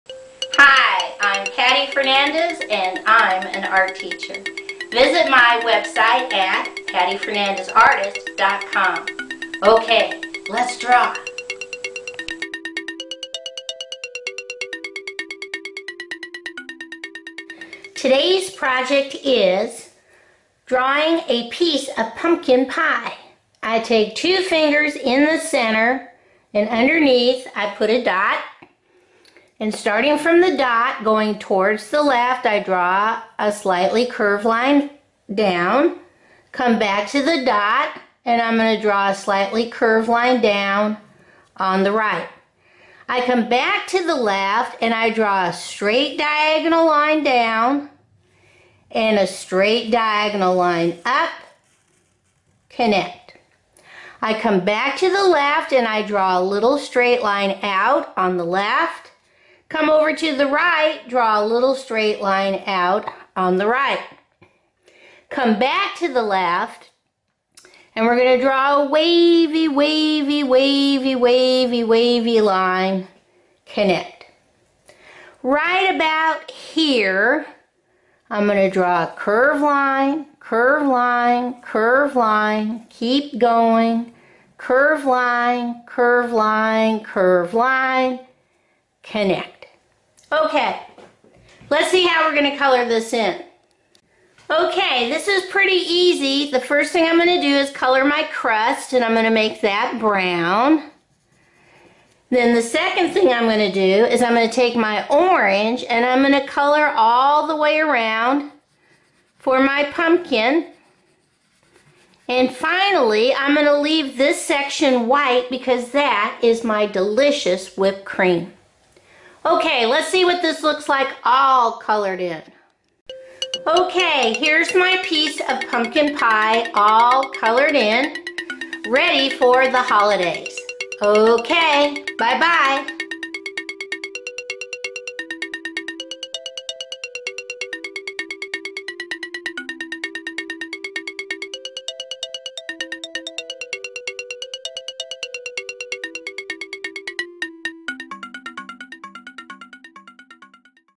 Let's Draw a Piece of Pumpkin Pie!
Easy to follow directions, using right brain drawing techniques, showing how to draw a piece of pumpkin pie.